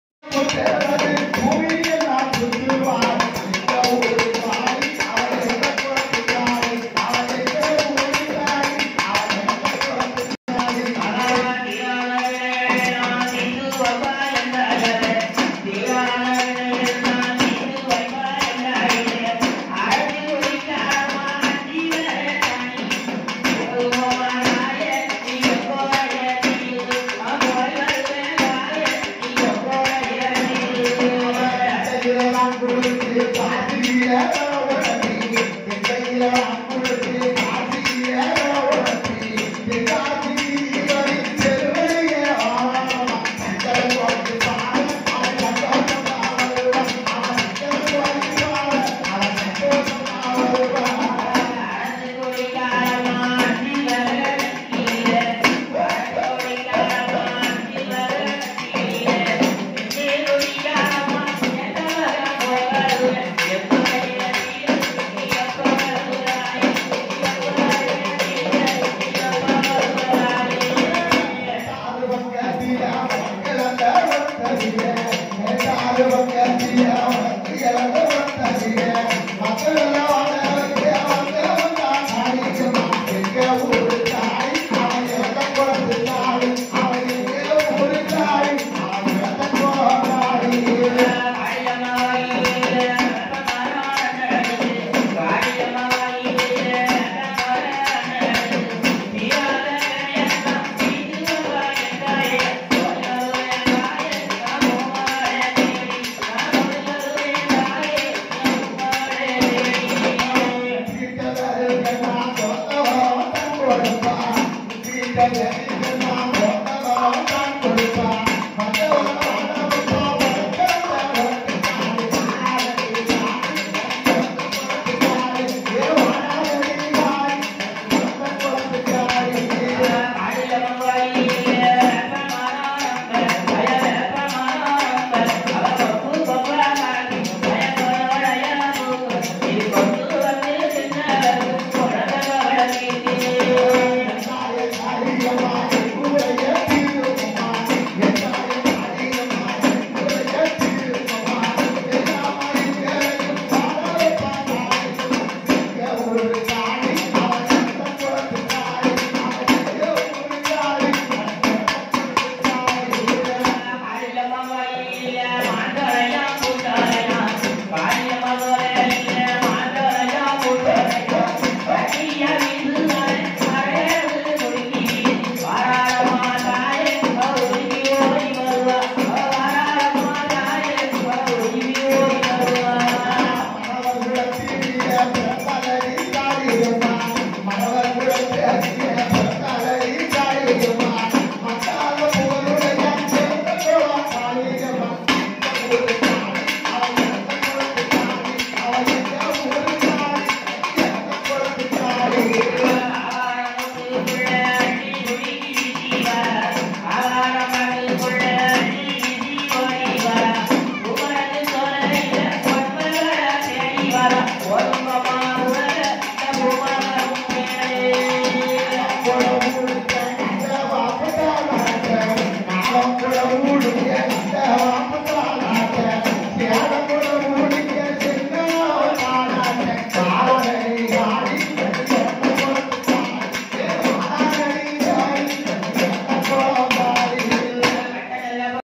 சித்திரை திருவிழா புதன்கிழமை நடந்த கும்மி பாட்டு
முளைப்பாரியை கரைப்பதற்கு முன்னாள் கோவிலில் கும்மி கொட்டி எப்போது